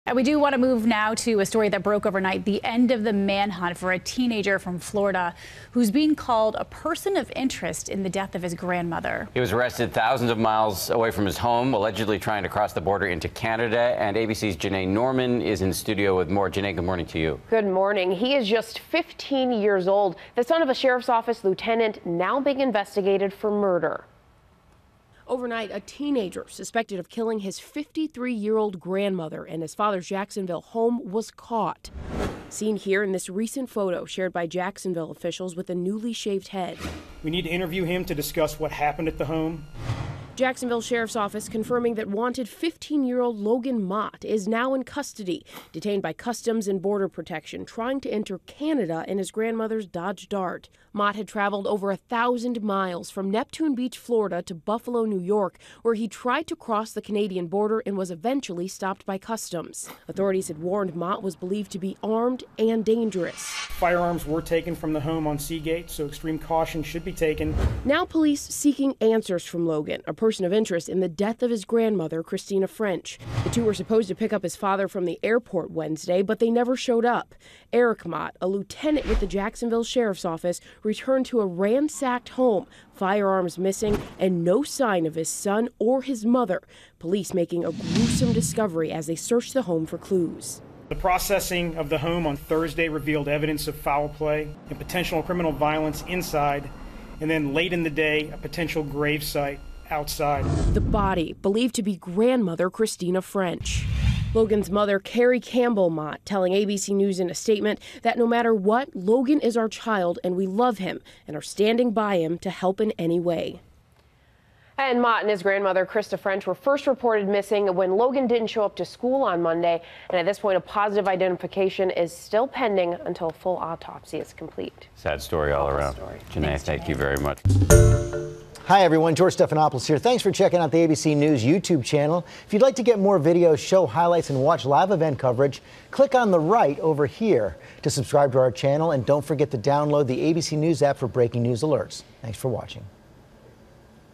Teen named a 'person of interest' in grandmother's death detained near Canadian border
The Jacksonville, Florida sheriff's office said the 15-year-old was in custody.